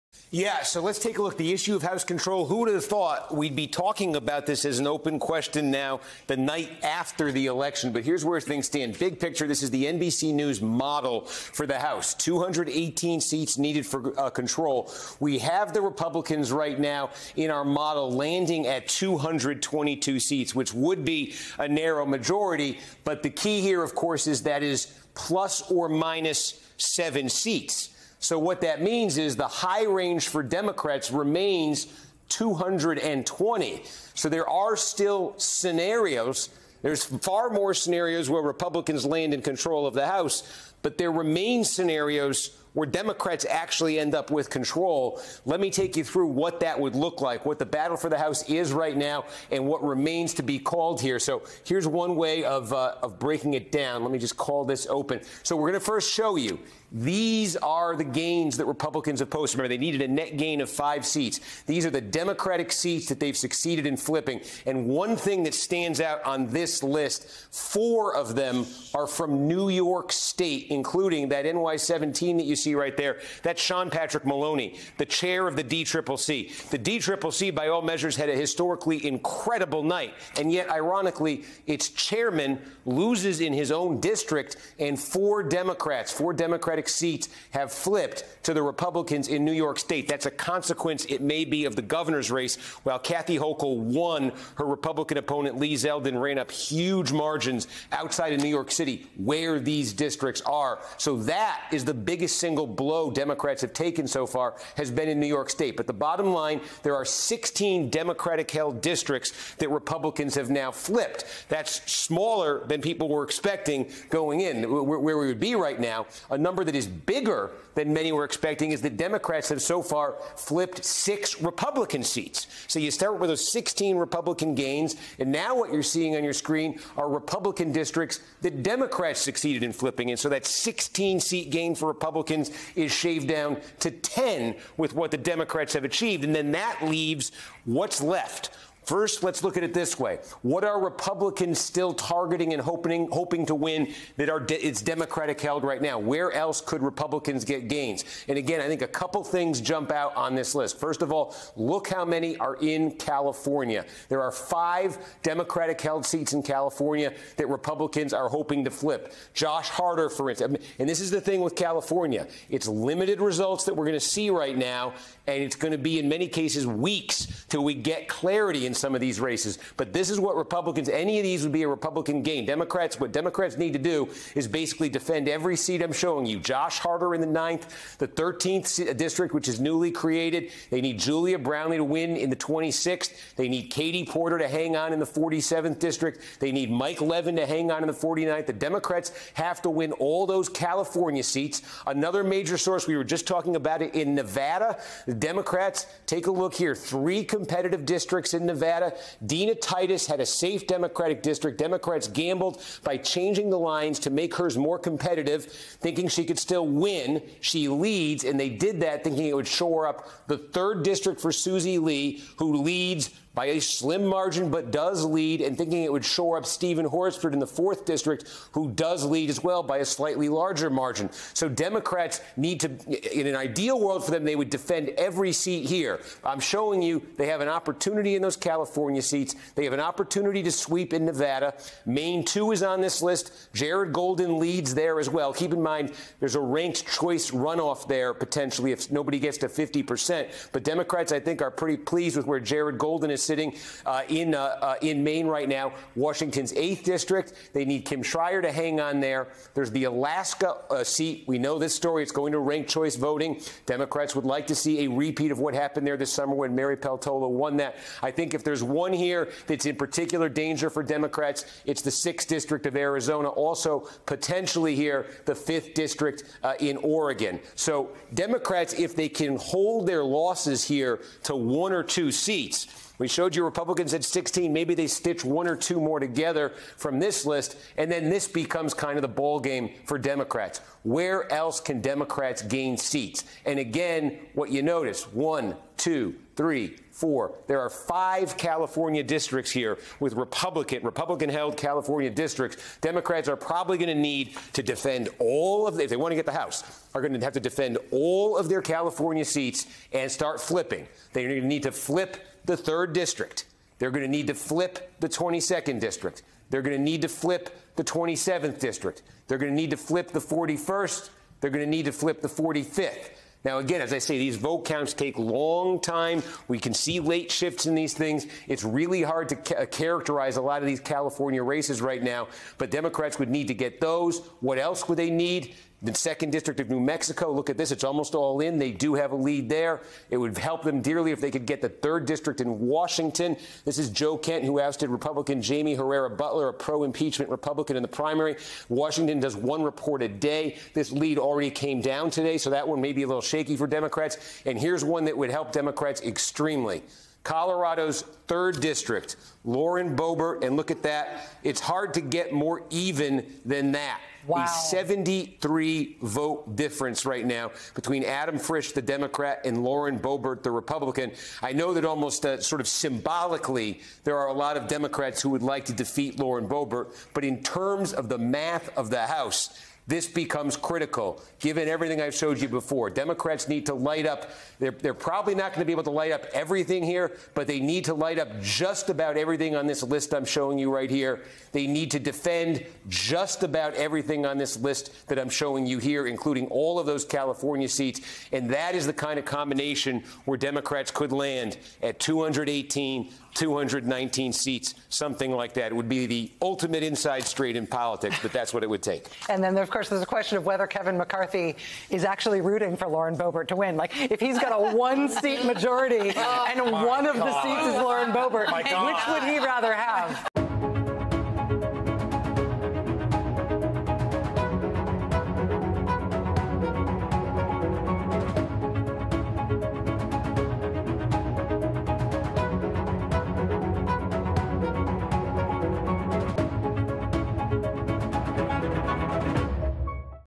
Uncalled Races Leave Paths For Both Parties To Control The House
Steve Kornacki outlines the House races that have yet to be called and the narrow math required for Democrats to retain control of the House and the best outcomes disappointed Republicans could hope for.

Ballots were cast and results of the 2022 midterm elections are still trickling in. Follow the most important races as House and Senate control still hangs in balance